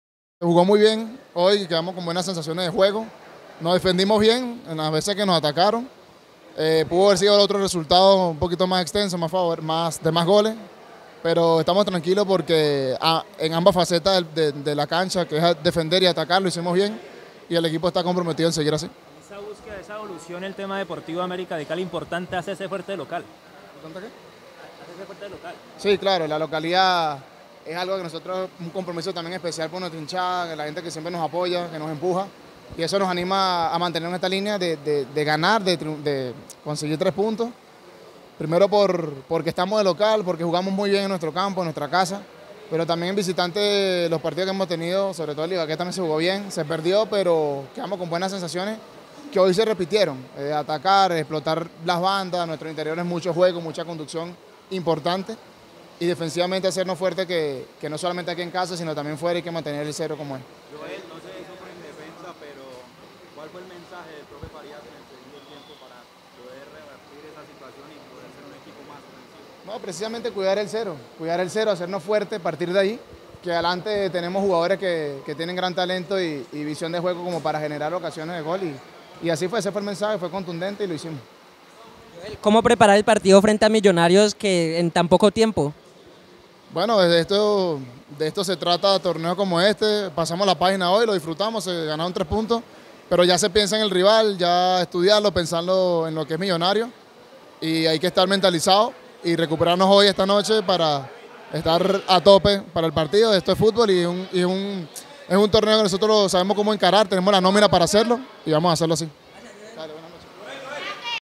Joel Graterol en Zona Mixta tras la victoria de América a Patriotas por la fecha 4
® Zona Libre De Humo - 2024